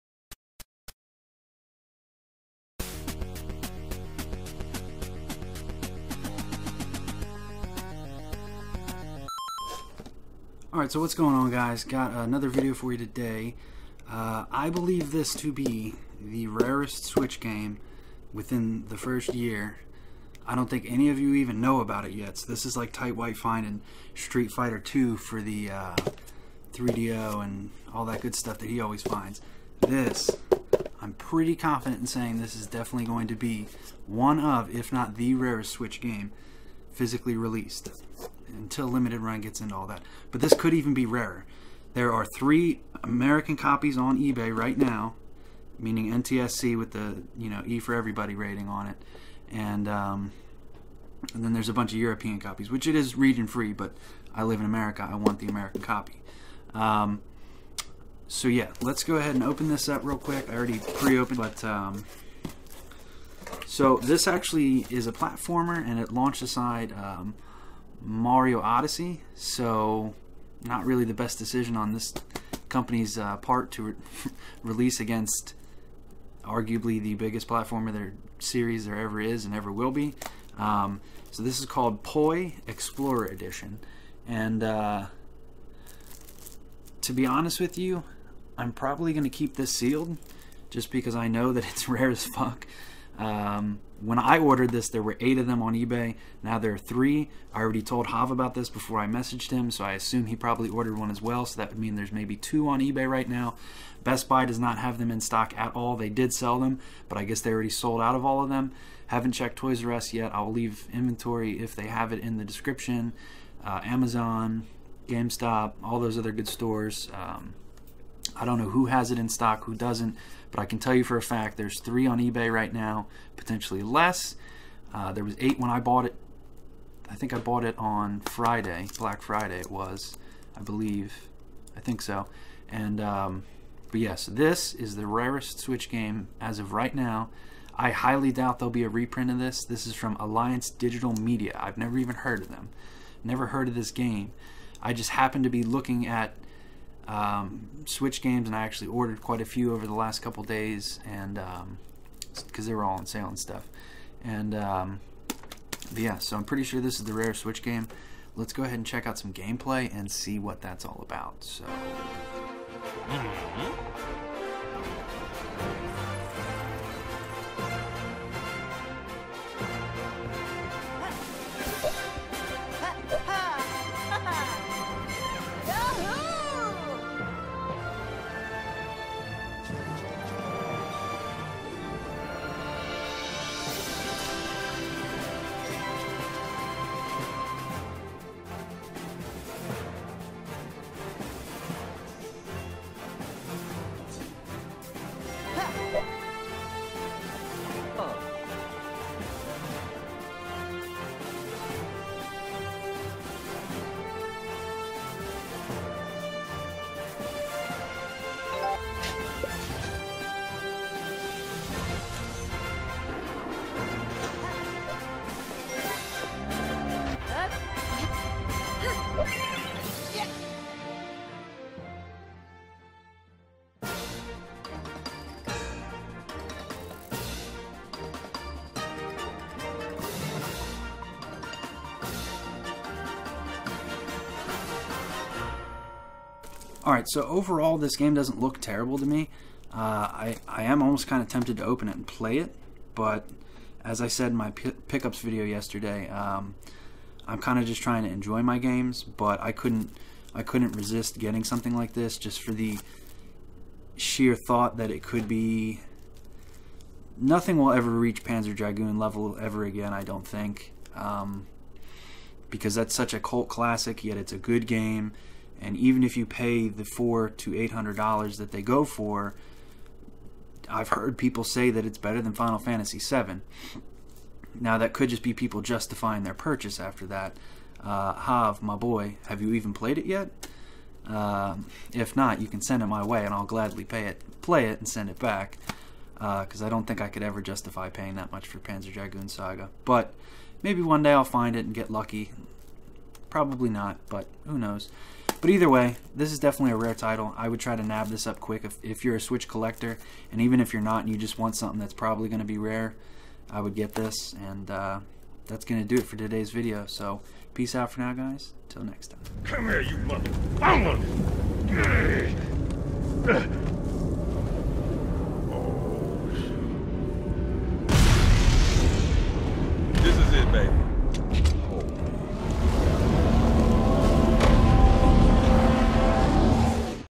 Switch Already Has A Rare Physical Games
Rare Nintendo Switch game poi. This is all speculation on my part i could always be wrong but i don't think i am.

Mail Can be sent here
ALEX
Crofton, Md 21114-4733